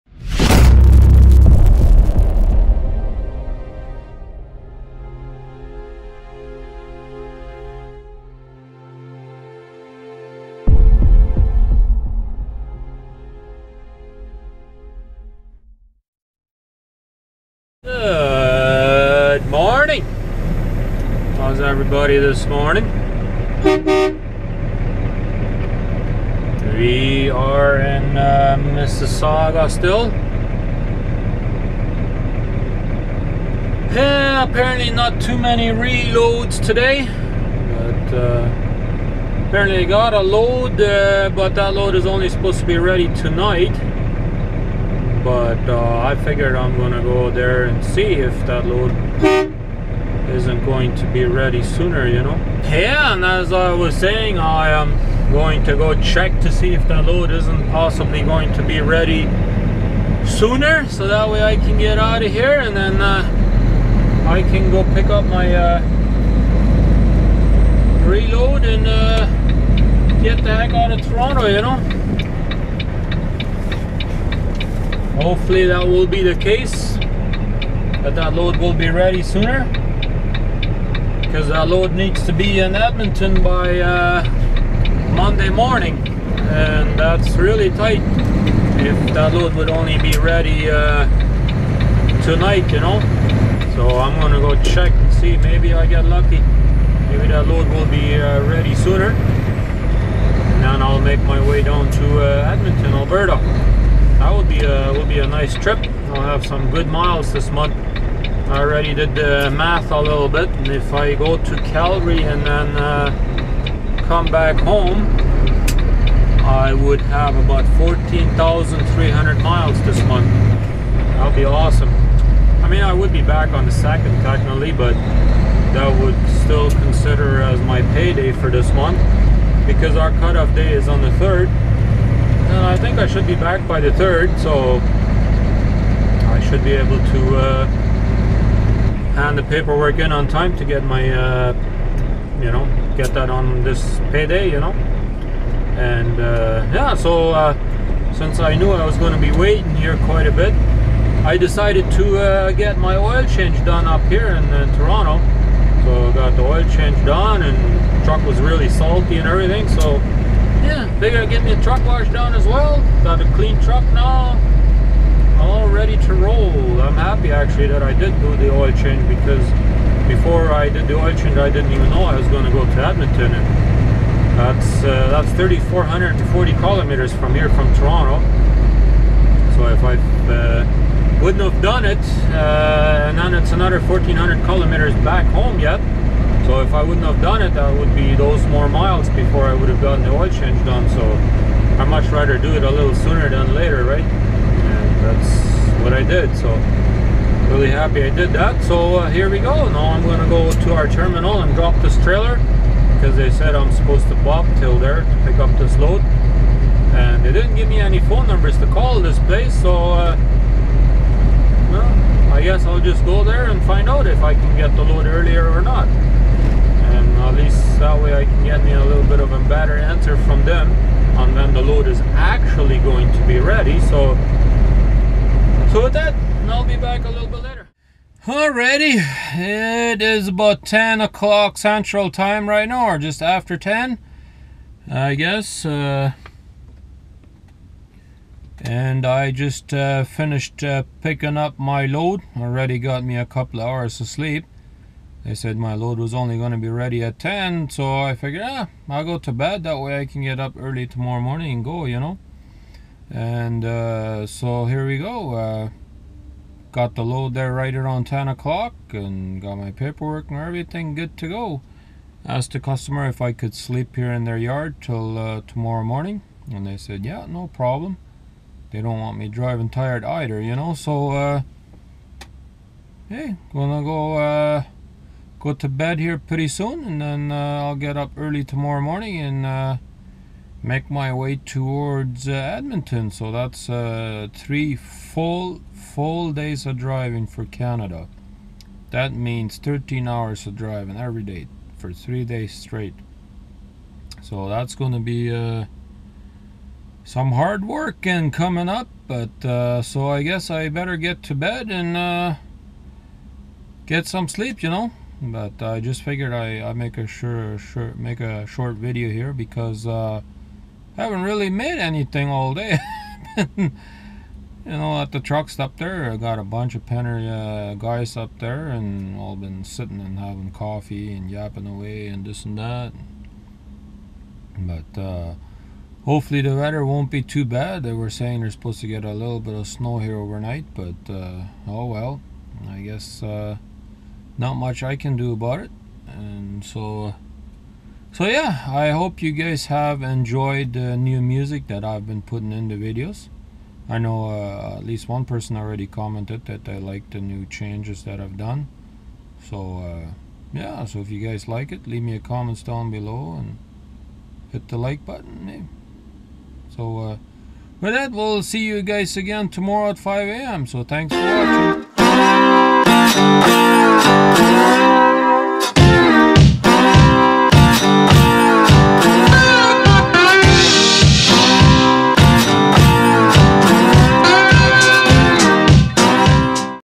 Trucker Rudi 11.27.14 no loads today so time off in Mississauga ON
no loads today so time off in Mississauga ON